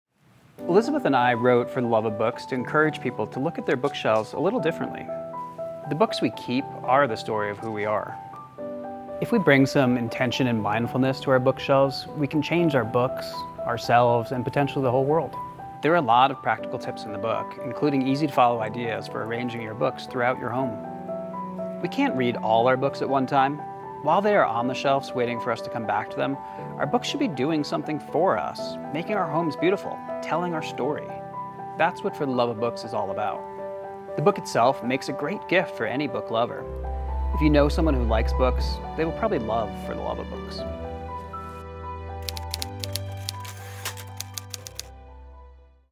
A Look Into "For the Love of Books" by Thatcher Wine & Elizabeth Lane | Juniper Books
For the Love of Books gathers all of Juniper Books Founder Thatcher Wine's philosophy about books, curating and designing libraries into one volume. This stunning coffee table book, published by Gibbs Smith, features photographs from our 18 year archive of library images, exquisite illustrations by Samantha Hahn, and insights and tips for displaying your love of books and telling your own story on your shelves from Thatcher and Elizabeth Lane.

For the Love of Books is about storytelling beyond the pages of our favorite books. Our books―the ones we choose to keep―tell the story of who we are. They remind us who we once were and who we aspire to be.